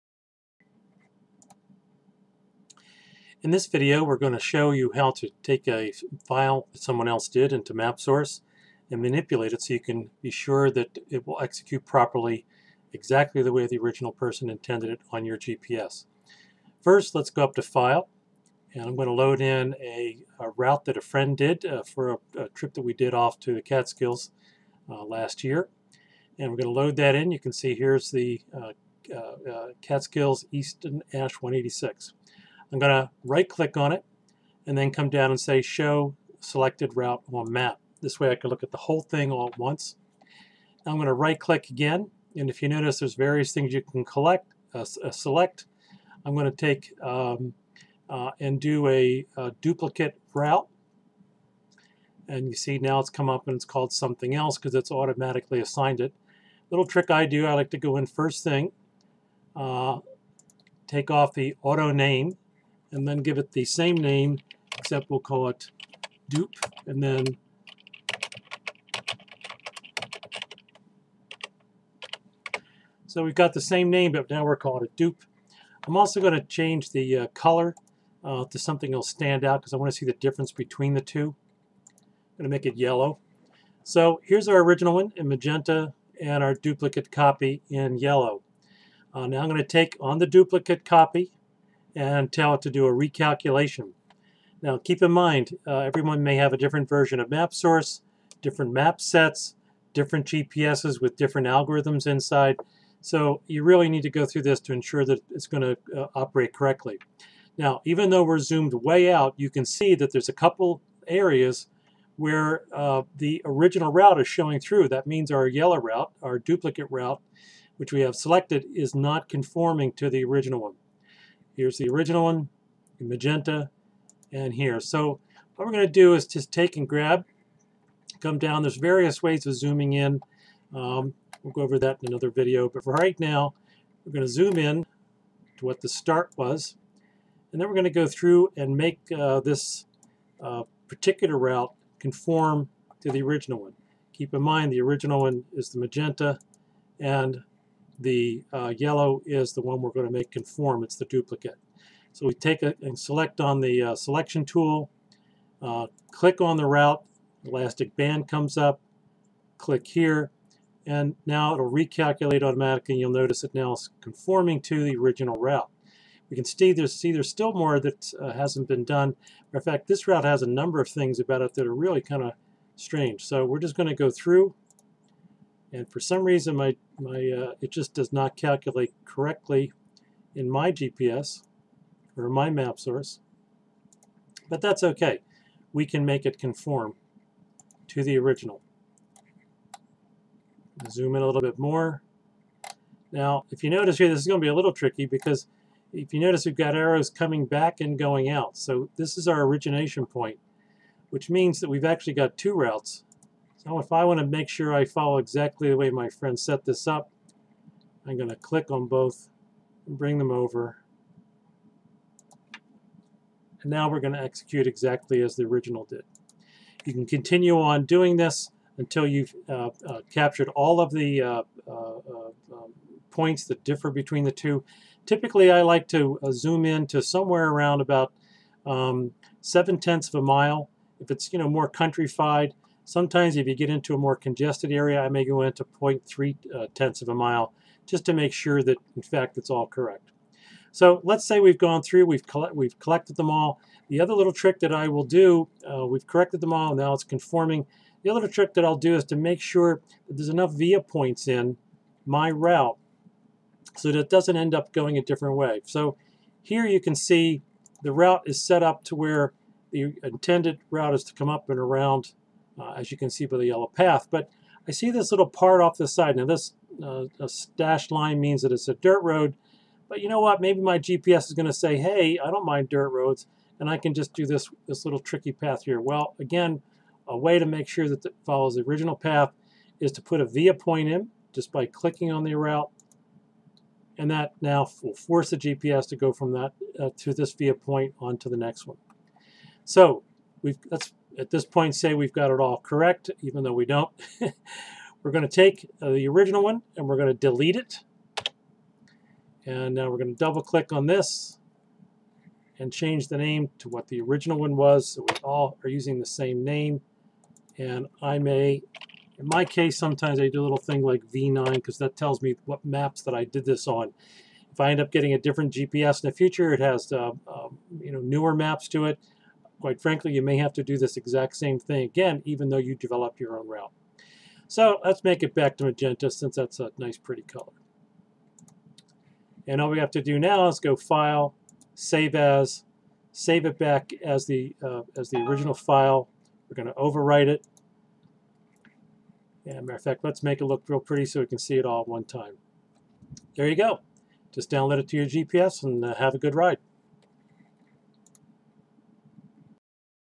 Garmin Mapsource - using someone else's route in your GPS
Demonstration using Garmin's Mapsource - how to take someone else's route and ensure it will exicute the route exactly as intended when in your GPS.